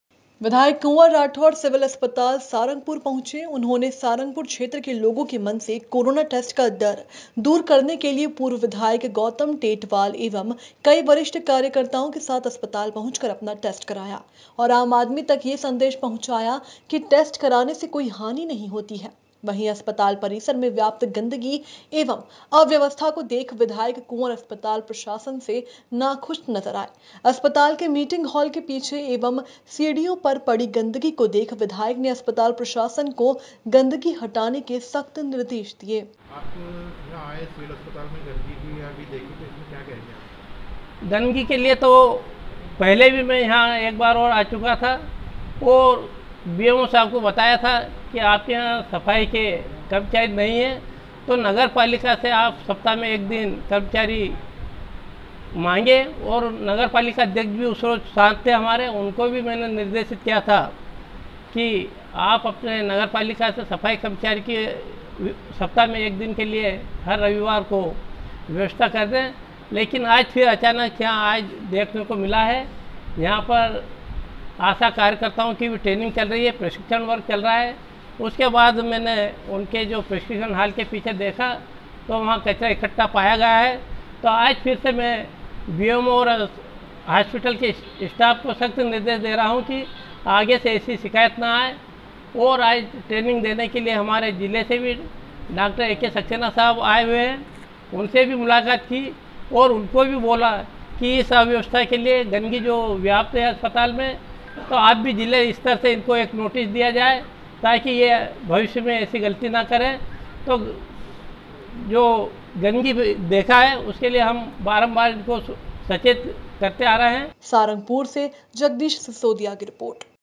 सारंगपुर-अस्पताल पहुंचे विधायक
विधायक कुंवर कोठर सिविल अस्पताल सारंगपुर पहुँचे।  उन्होंने सारंगपुर क्षेत्र के लोगो के मन से कोरोना टेस्ट का डर दूर करने के लिये पूर्व विधायक गौतम टेटवाल एवम् कई वरिष्ठ कार्यकर्ताओ के साथ अस्पताल पहुँच कर अपना टेस्ट कराया और  आम आदमी तक ये संदेश पहुँचाया  कि टेस्ट कराने से कोई हानि नहीं होती। वही अस्पताल परिसर में व्याप्त गन्दगी एवम् अव्यवस्था को देख विधायक कुंवर  अस्पताल प्रशासन  से नाखुश नजर आये । अस्पताल के मिटिंंग हाल के पीछे एवम् सिड़ीयो पर  पड़ी गन्दगी को देख विधायक ने अस्पताल प्रशासन को गन्दगी हटाने के सख्त निर्देश दिया ।